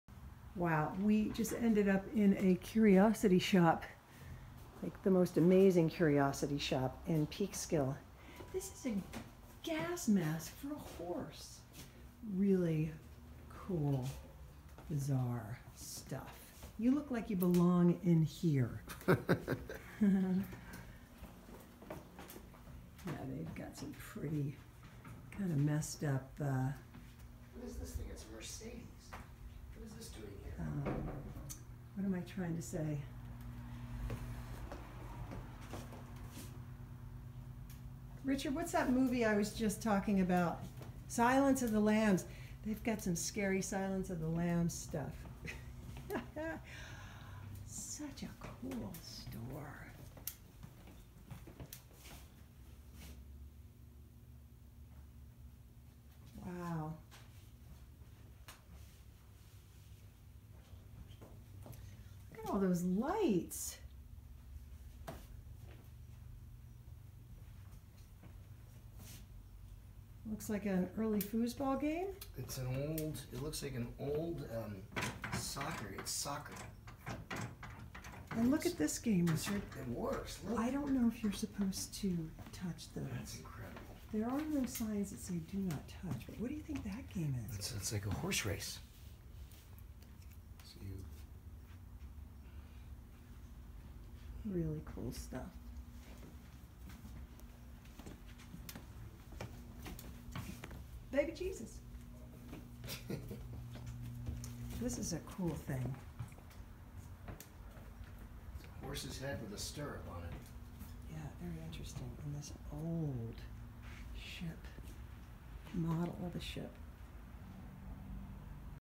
Exploring Early Electrics, Peekskill New York
Wow! More than any lighting store we have ever seen! Their unique and curious collections of antiques was fascinating!